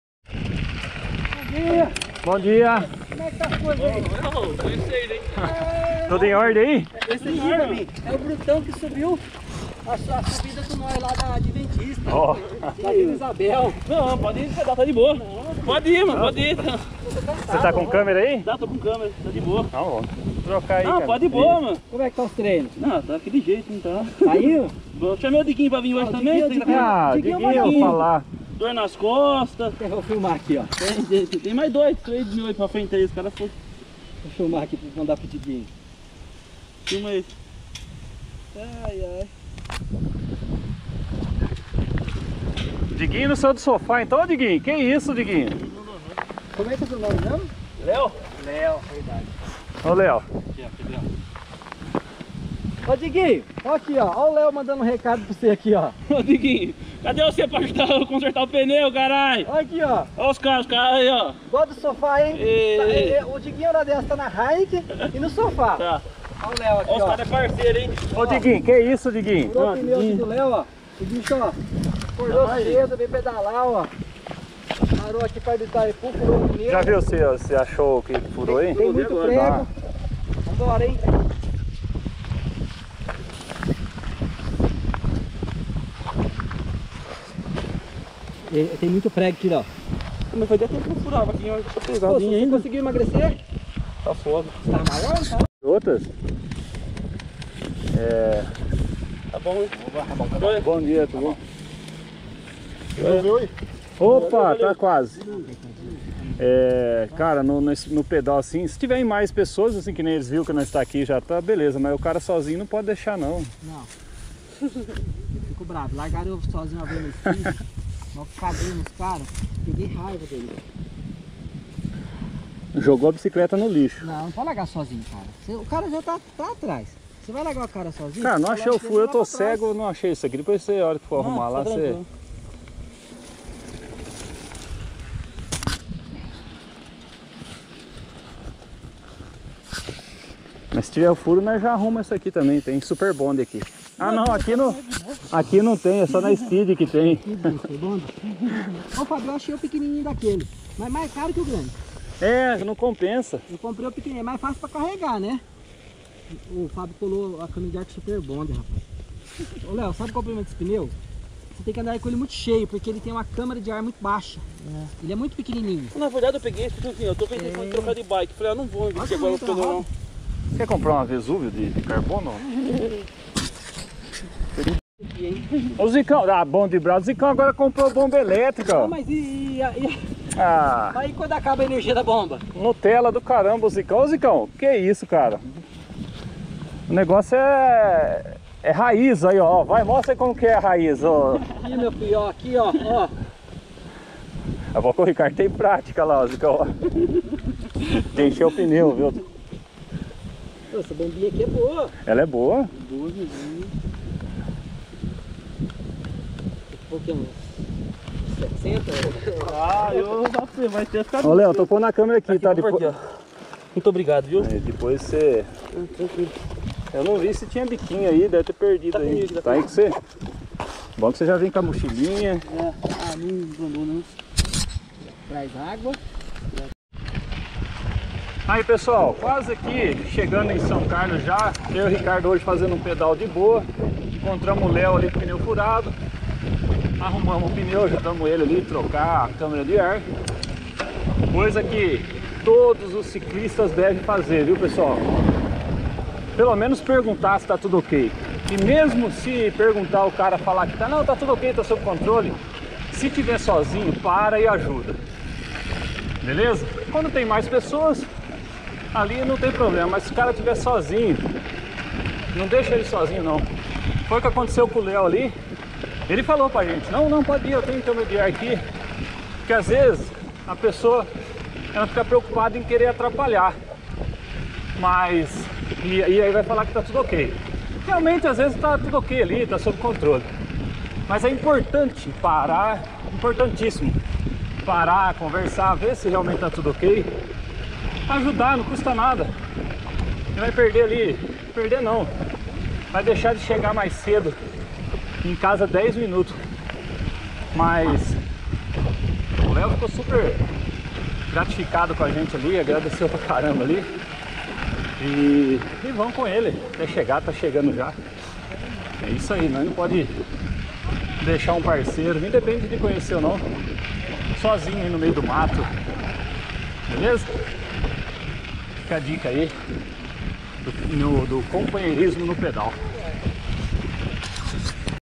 Nunca deixe de ajudar
Companheirismo no pedal, é muito importante ajudar alguém que esteja sozinho.
sempre que perguntar, pare e veja se realmente está tudo ok e não deixe alguém sozinho.